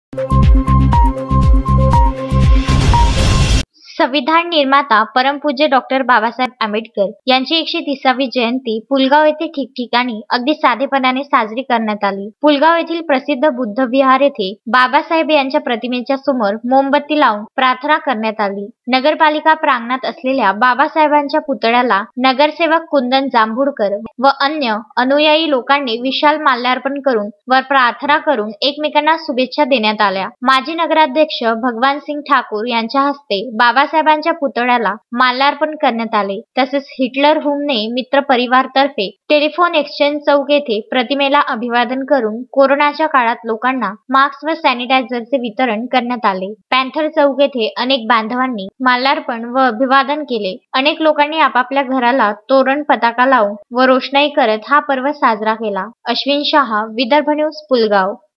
Pulgaon | डाॅ बाबासाहेब आंबेडकर यांची 130 वी जयंती साजरी
डाॅ बाबासाहेब आंबेडकर यांची 130 वी जयंती साजरी

कोरोनो संकट लक्षात घेता मास्क व सॅनिटायझर वितरीत